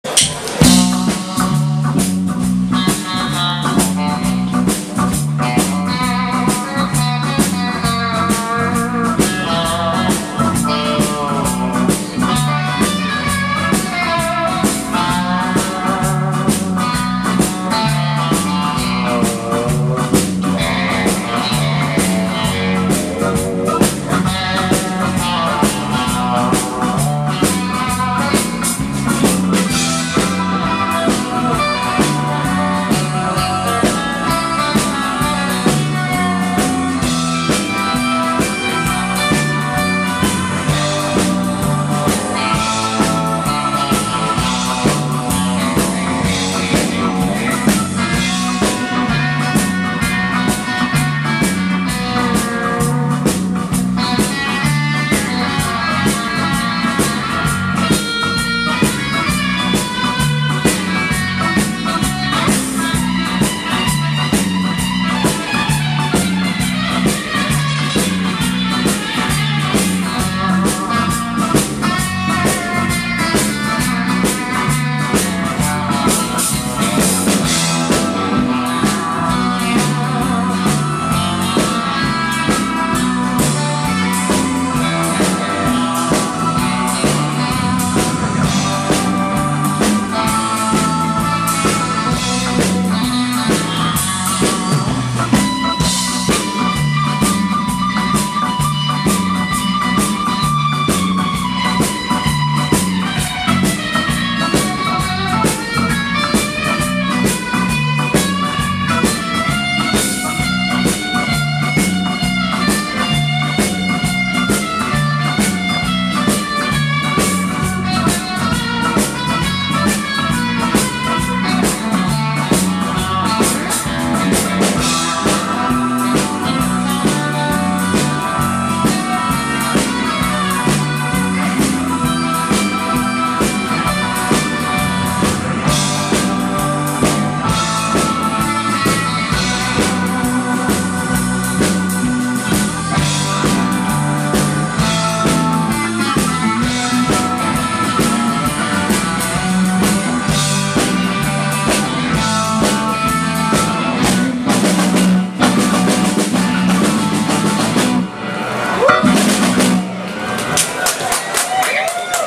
Lefthand Freddy [1] (Nuenen, Blues'm, 19-5-2024)
Blues'm in Nuenen is een jaarlijks openluchtfestival met bluesmuziek.